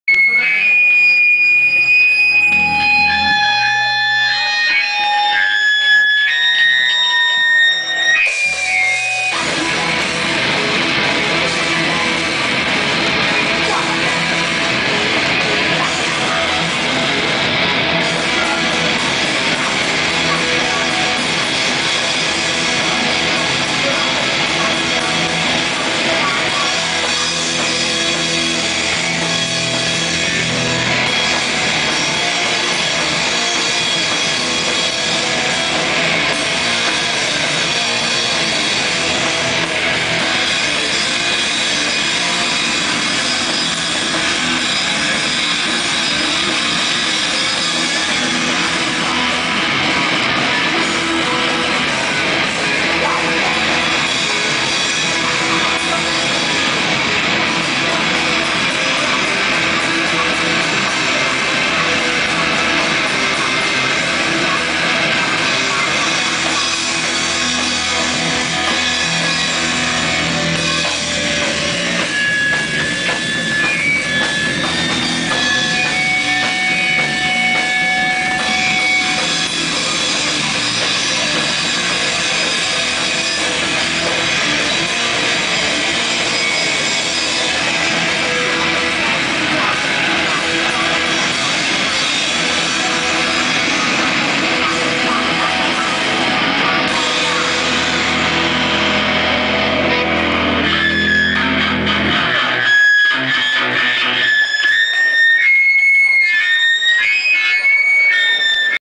INSERVIBLES @ Etengabeko Zarata Jaialdia #9, 08/10/1
DIY-Subactitud Channel
Inservibles en la edición num. 9 del clasico festi de ruido Etengabeko Zarata que como todos los años se celebra en el Kultur Etxea de Altza. Muy buena banda mexicana que sorprendio a mas de unos con su punk sucio y ruidoso. Una de las agradable sorpresas de la noche.